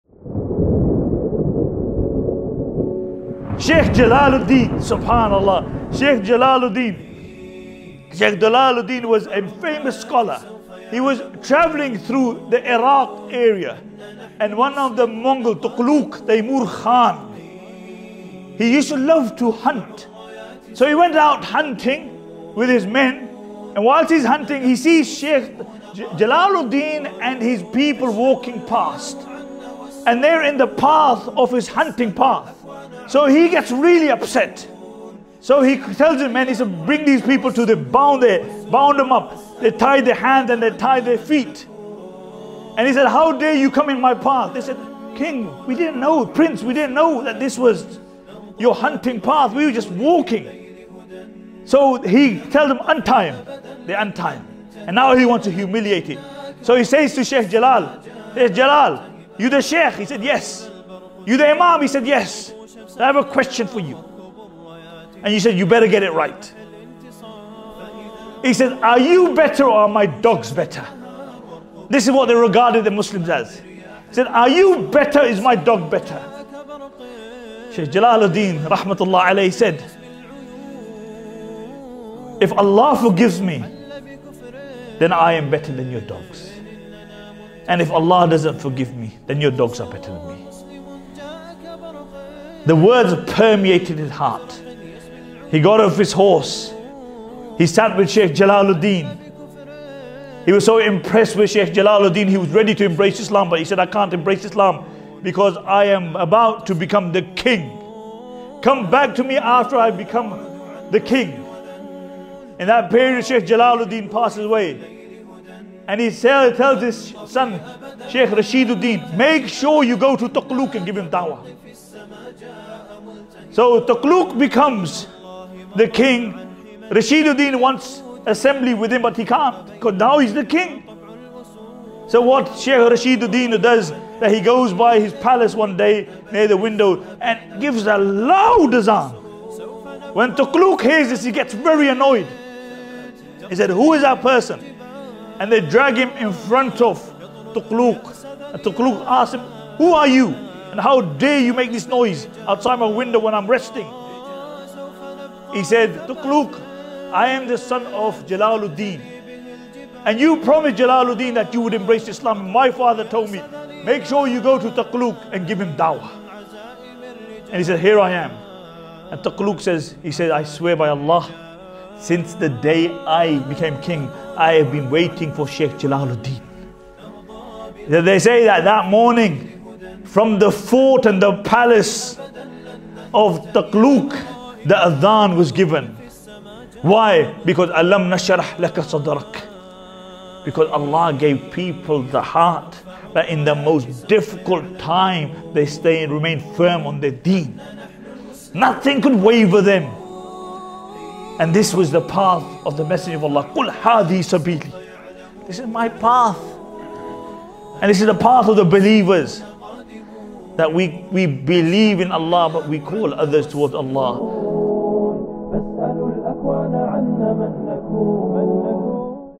Dawah of Shaykh Jalal Al-Din to Mongol Tughlugh | Shaykh Zahir Mahmood
Featuring: Shaykh Zahir Mahmood